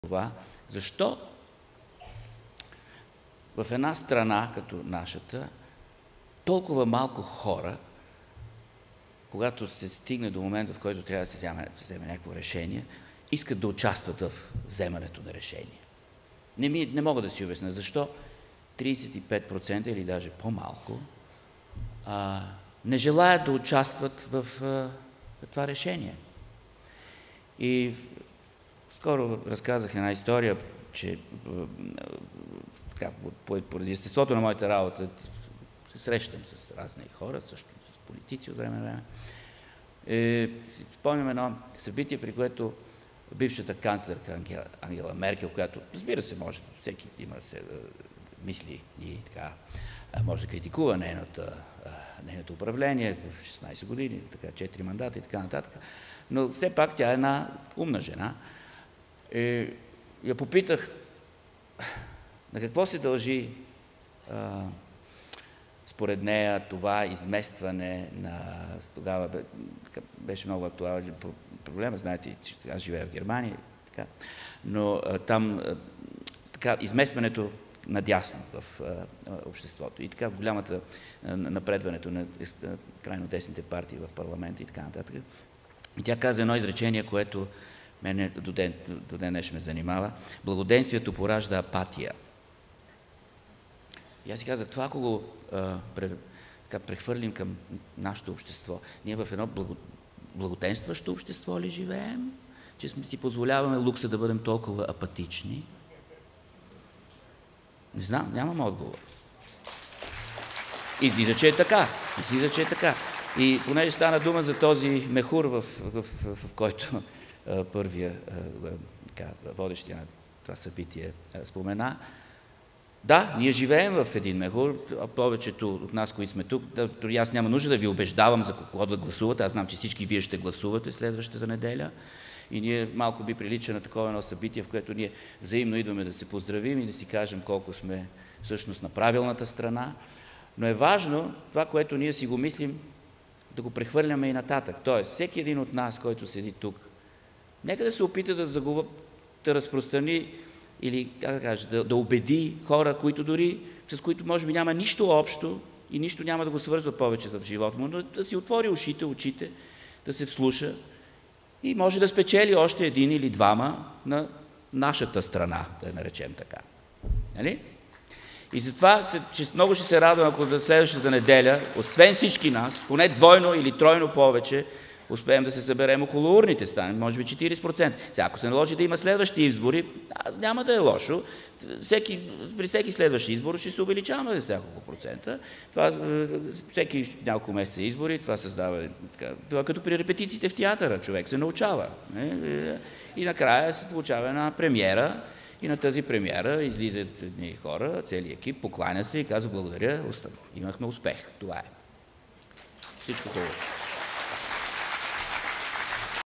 Чуйте призива на Самуел Финци да събудим България!
🌱На 27 октомври гласувайте с номер 2️⃣6️⃣
За България, в която корупцията намалява, а населението се увеличава
Последвайте ни!